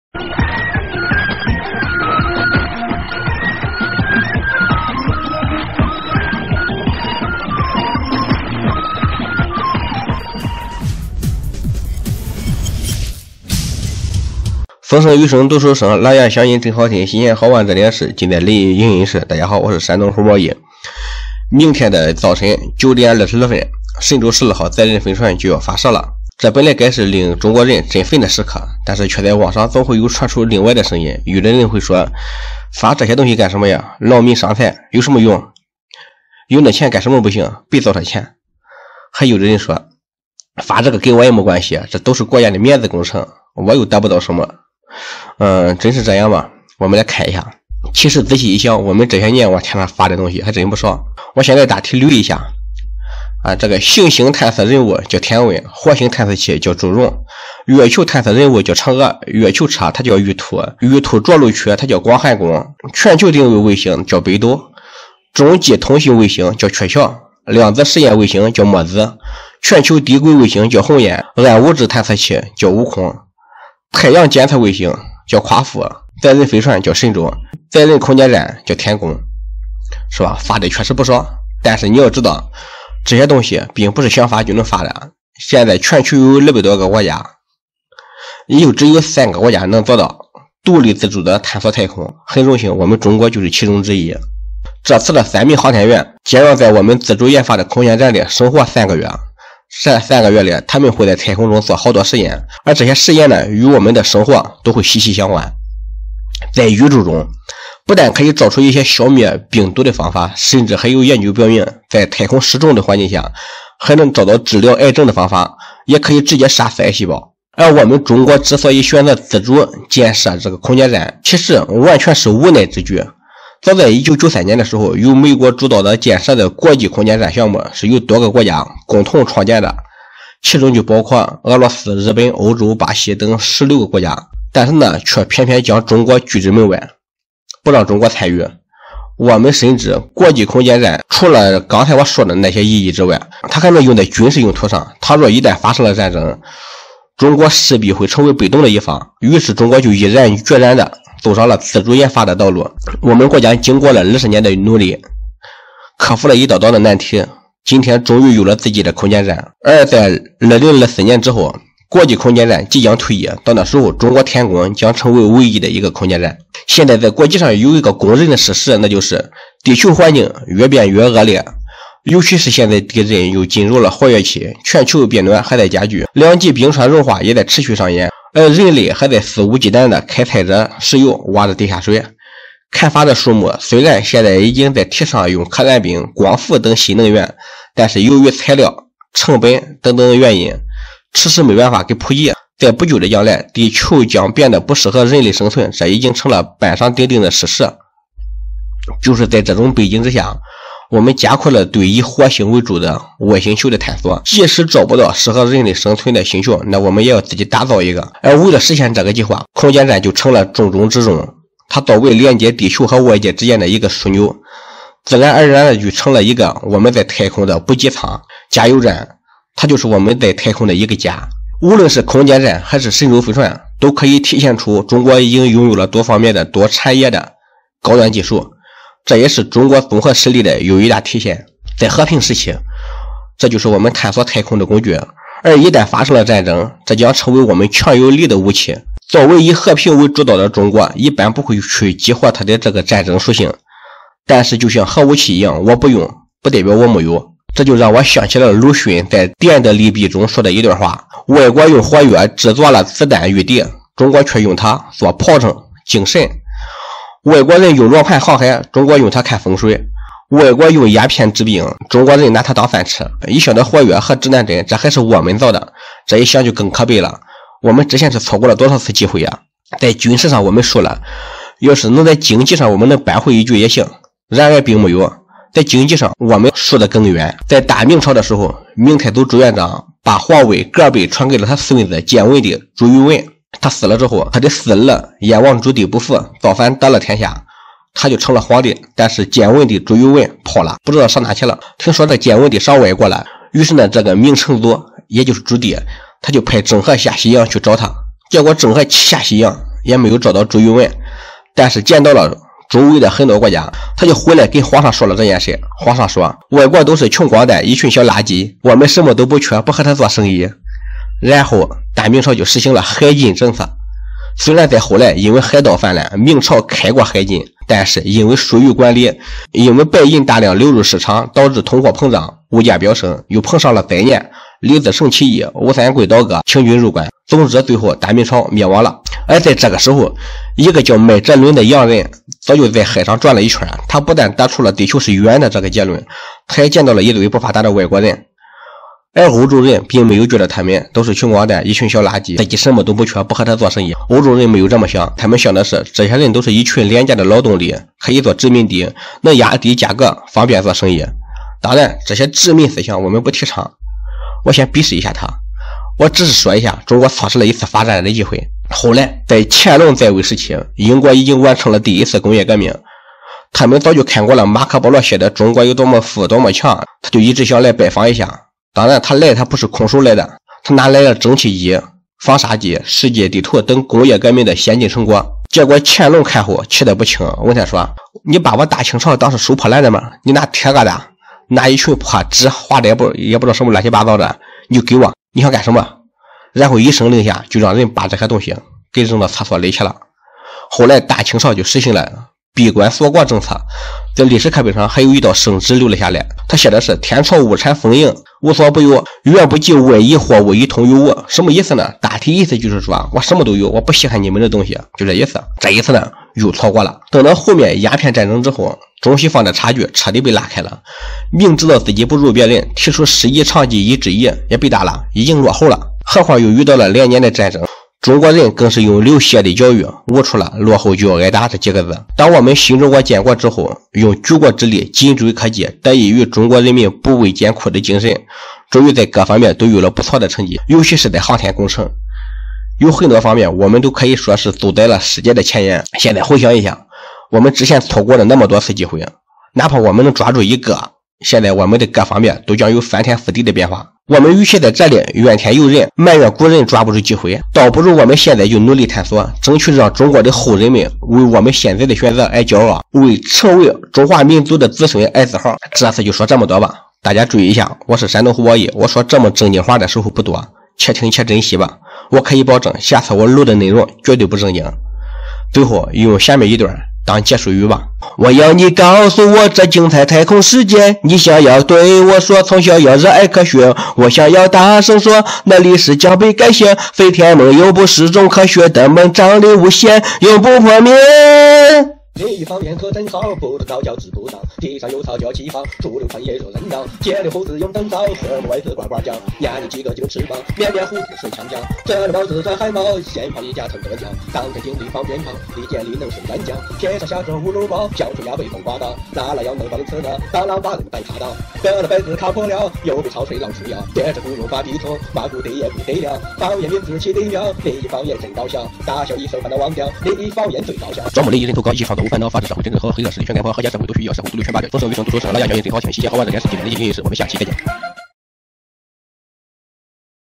歪歌社团 神舟十二号发射的有意义吗？是不是劳民伤财？听临沂人跟你说
明天早晨9点22分，神舟十二号载人飞船就要发射了，这本来该是令中国人振奋的时刻，但却在网上总会传出另外的声音，有的会说，发射这些东西干什么，劳民伤财，有什么用？有那钱干什么不行，白糟蹋钱……